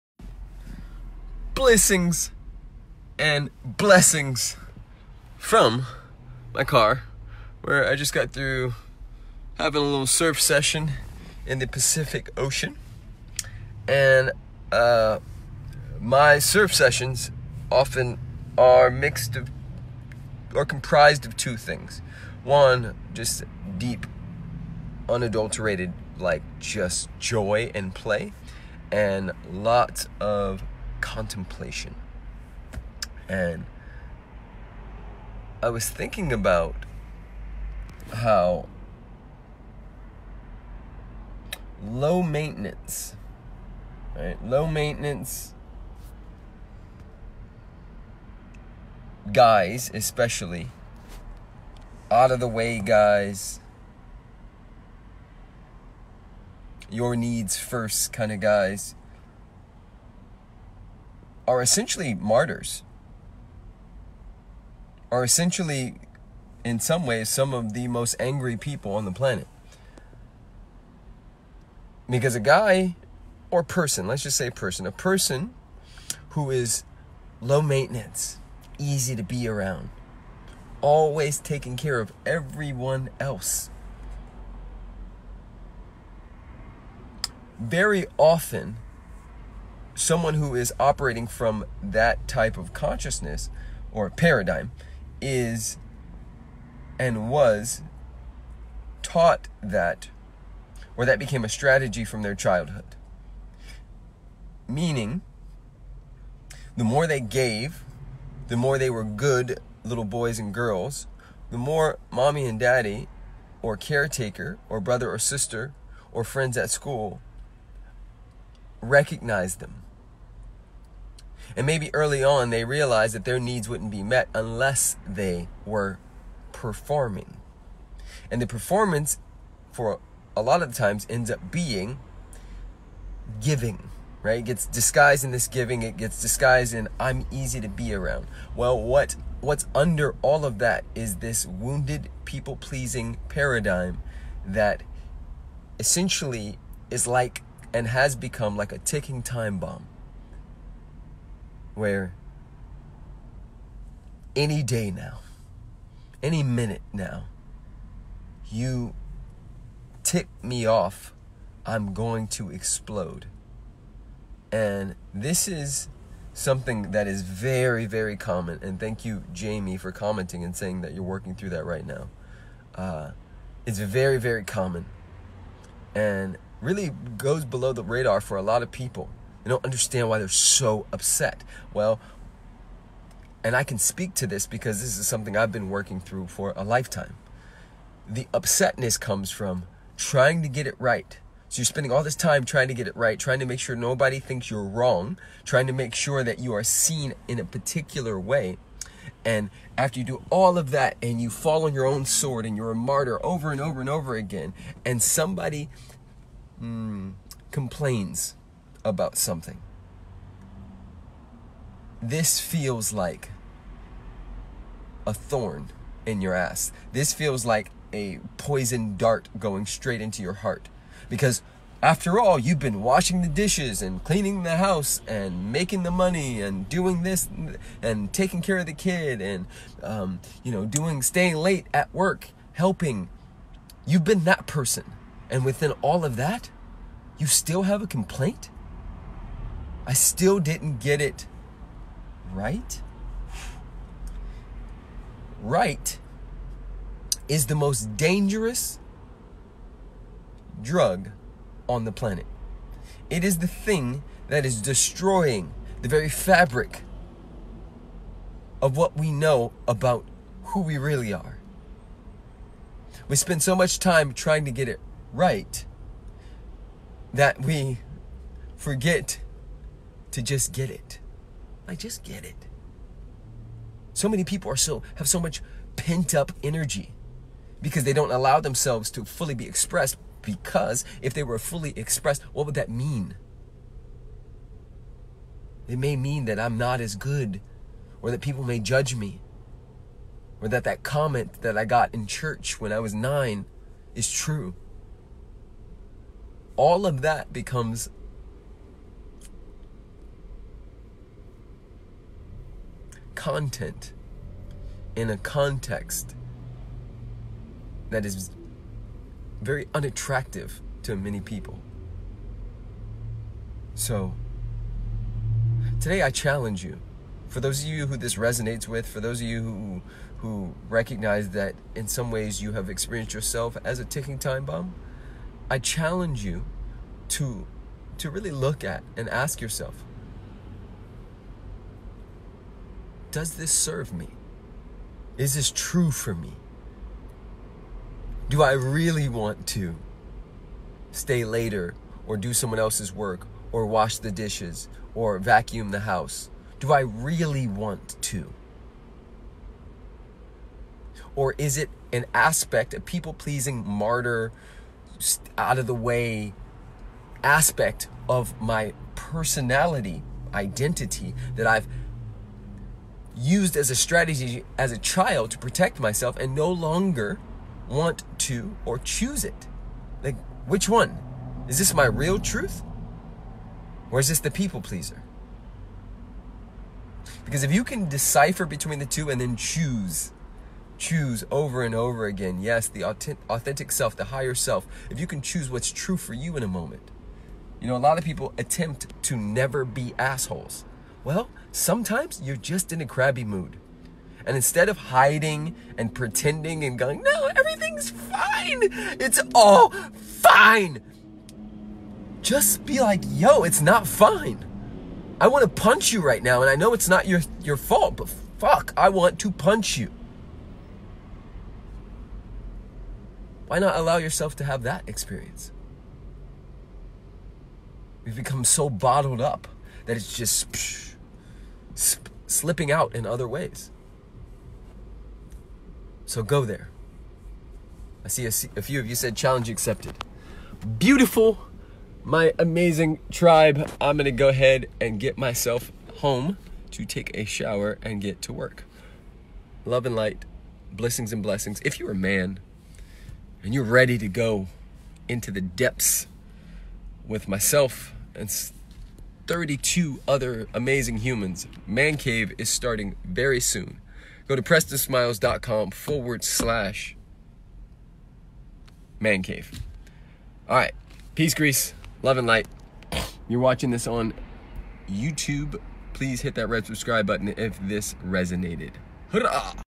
ANGRY 😡Being LOW MAINTENANCE is BS! | Preston Smiles
Most people have childhood wounds that cause them to be "easy" to be around...we call them low maintenance. However these are some of the most angry people...